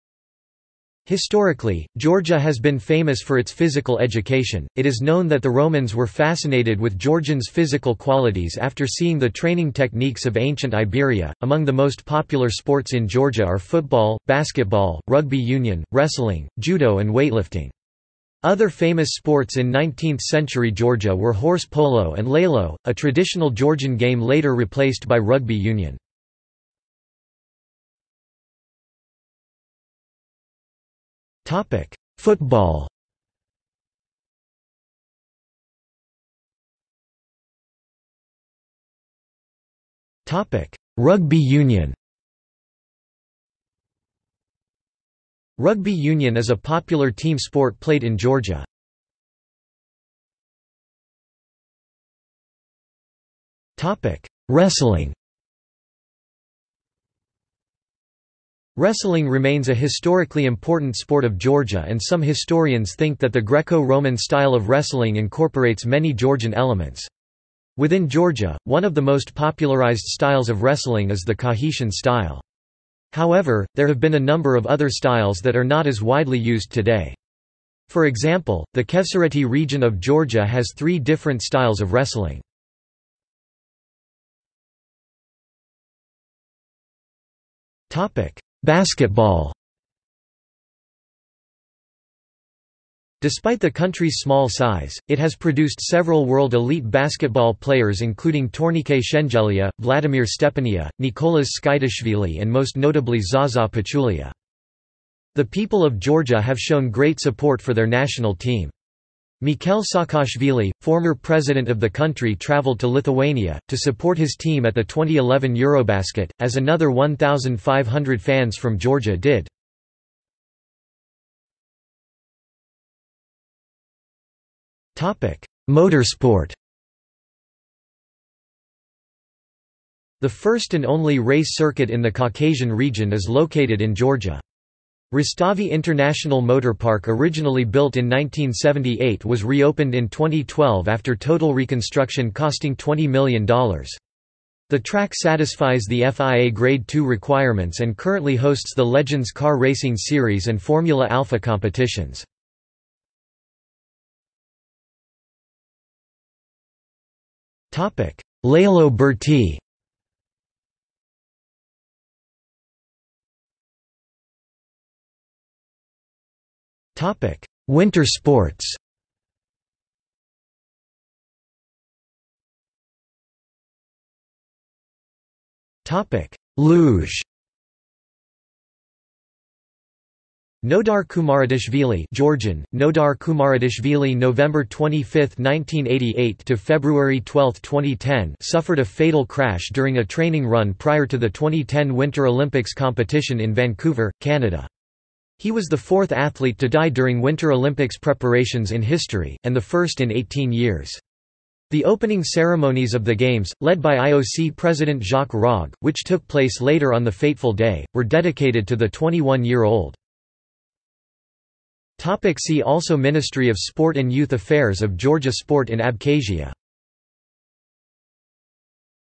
Sport in Georgia | Wikipedia audio article
This is an audio version of the Wikipedia Article:
Sport in Georgia

00:00:27 1 Football
00:00:36 2 Rugby union
00:00:50 3 Wrestling
00:01:24 4 Basketball
00:02:01 5 Motorsport
00:02:34 6 Lelo burti
00:02:43 7 Winter sports
00:02:52 7.1 Luge
00:03:31 8 See also

- Socrates

SUMMARY
Historically, Georgia has been famous for its physical education; it is known that the Romans were fascinated with Georgians' physical qualities after seeing the training techniques of ancient Iberia.Among the most popular sports in Georgia are football, basketball, rugby union, wrestling, judo and weightlifting. Other famous sports in 19th-century Georgia were horse polo and lelo, a traditional Georgian game later replaced by rugby union.